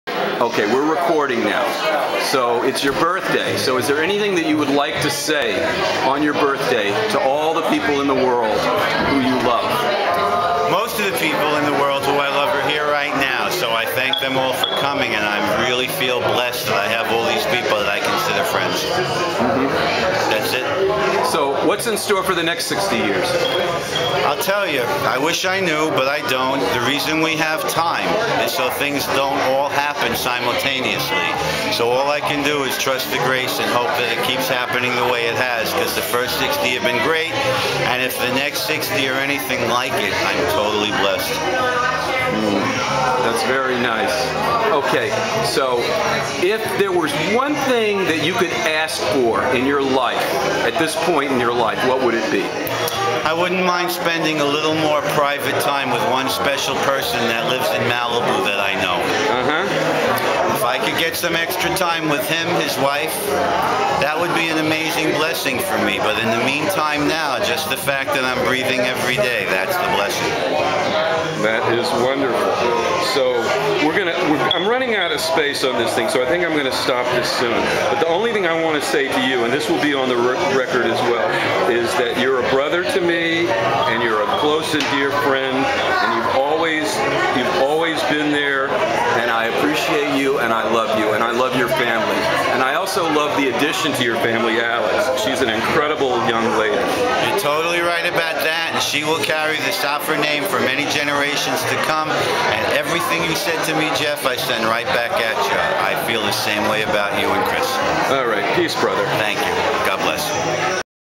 Sof at 60
Words of wisdom from the good doctor at his birthday party